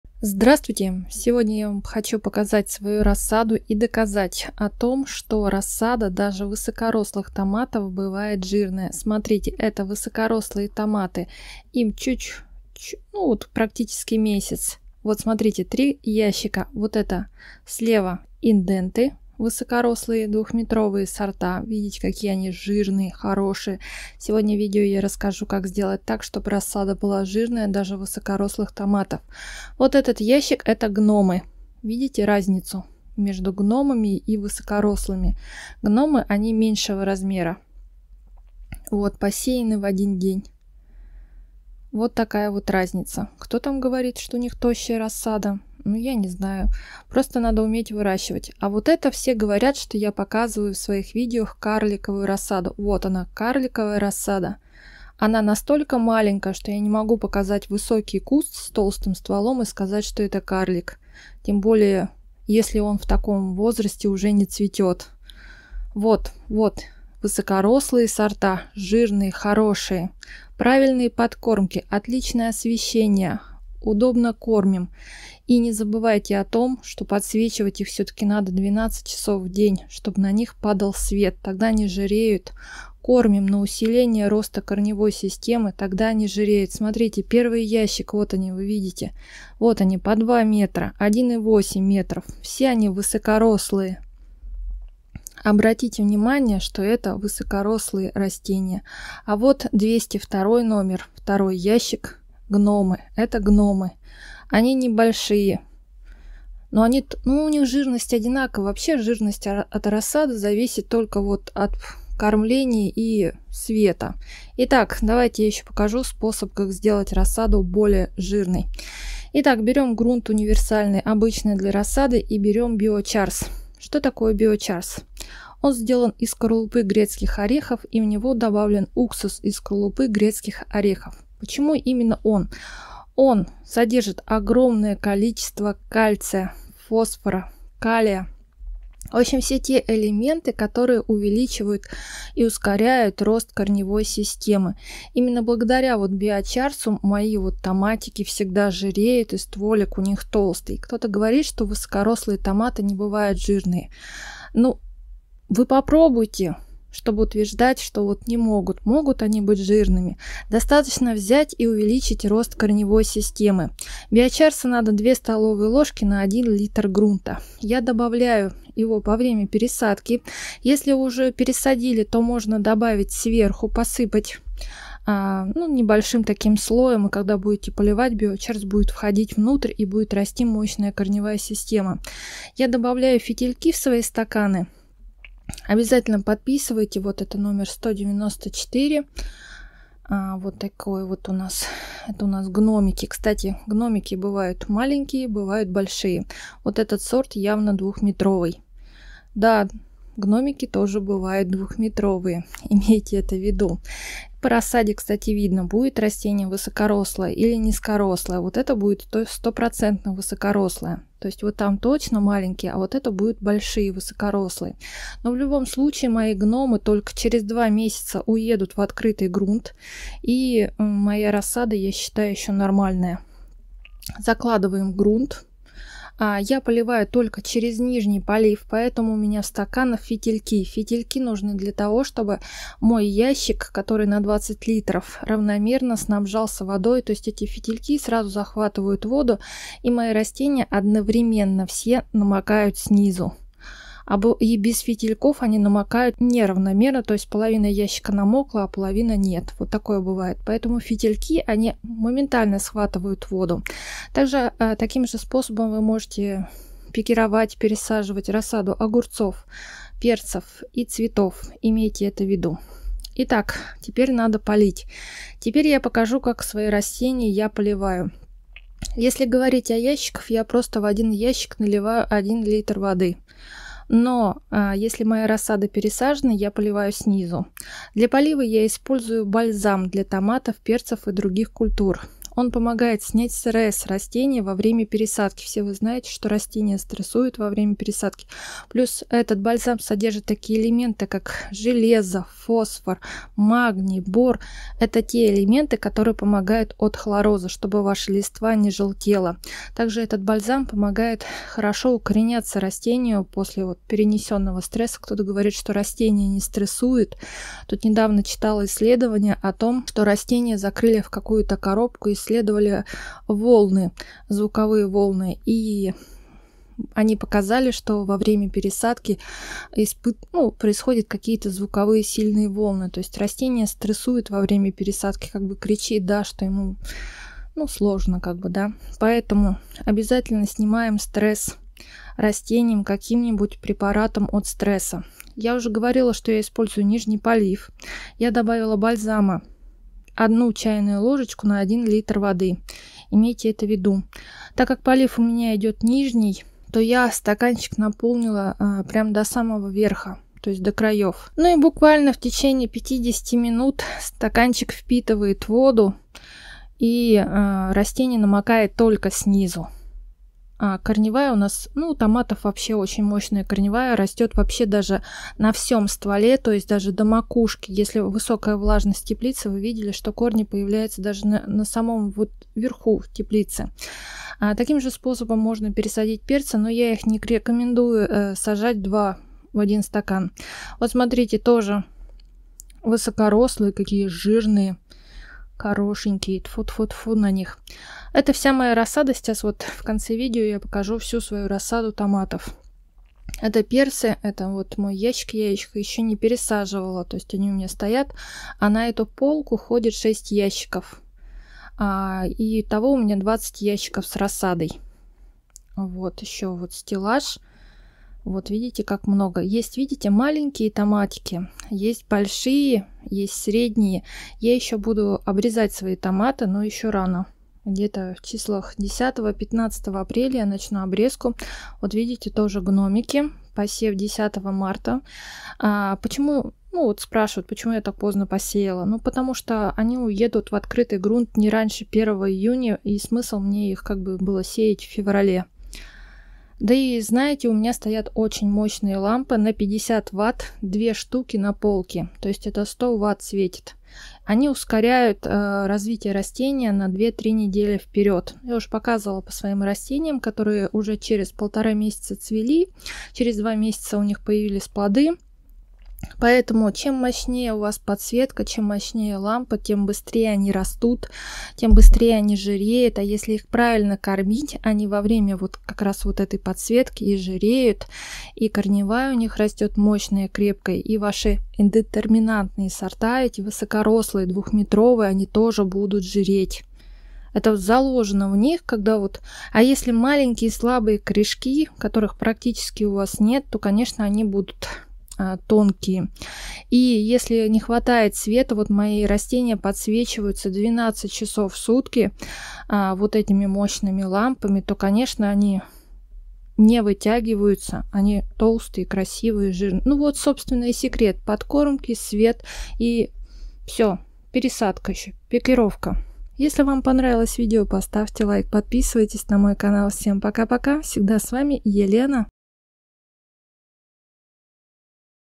Рассада ЖИРЕЕТ на глазах за 3 дня, три секрета толстой, мощной рассады
Показываю всю свою рассаду и рассказываю, что применяю, чтобы высокорослые томаты были с жирным стволиком.

Реклама: ИНН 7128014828, ООО "Шахкоал", 2VtzqvYN2mt